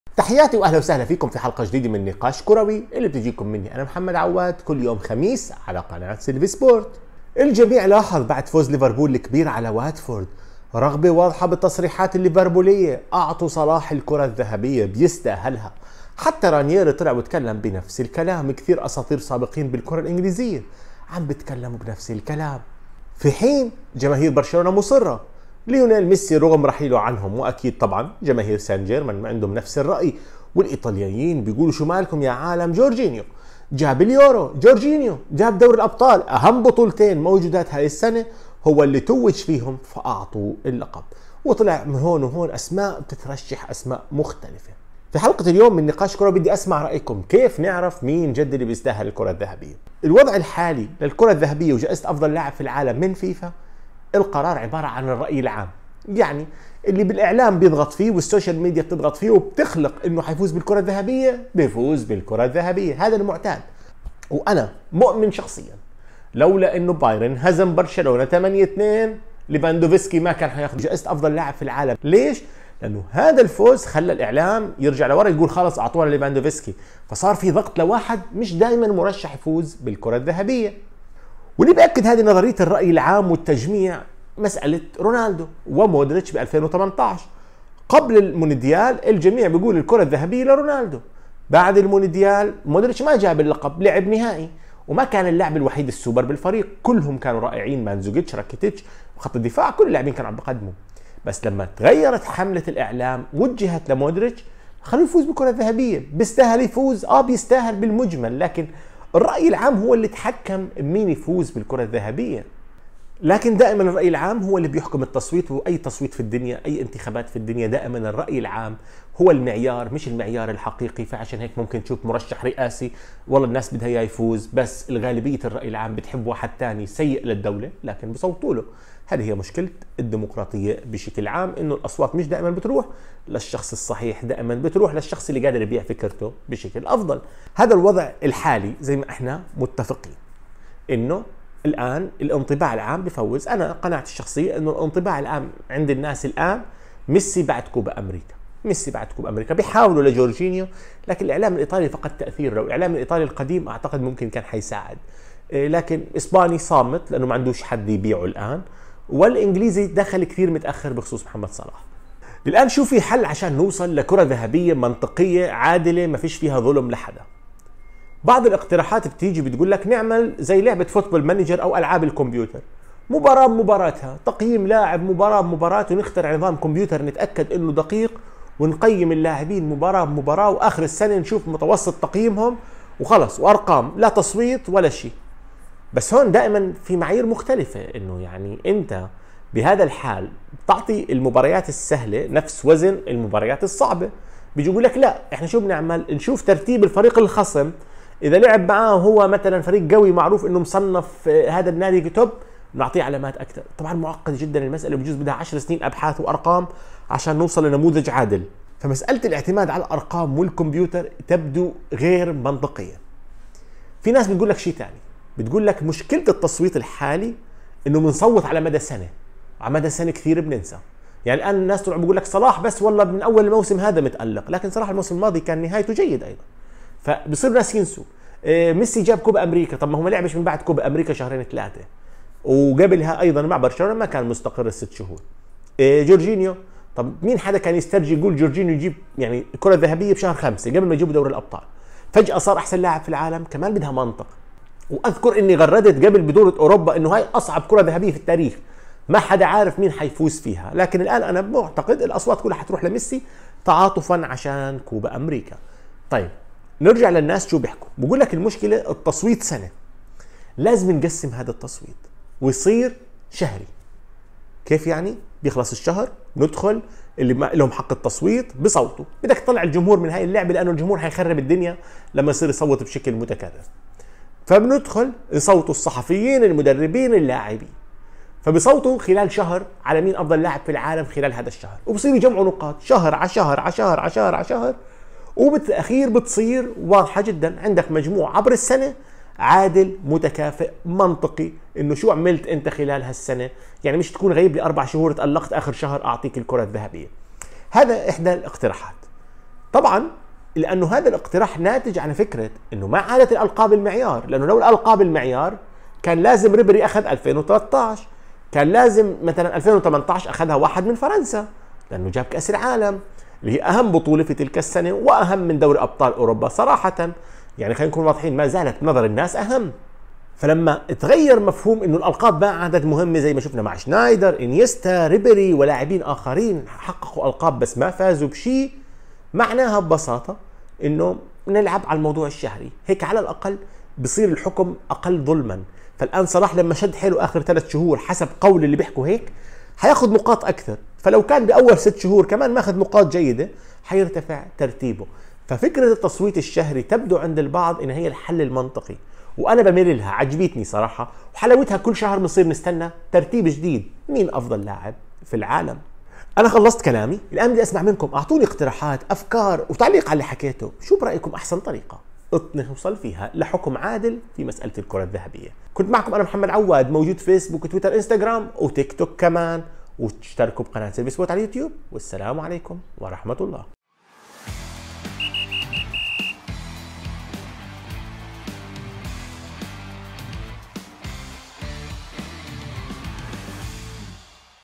أفضل طريقة لاختيار أفضل لاعب في العالم و الكرة الذهبية
ما هي أفضل طريقة لاختيار أفضل لاعب في العالم والفائز بالكرة الذهبية؟

حساباتي في شبكات التواصل:
تيلغرام :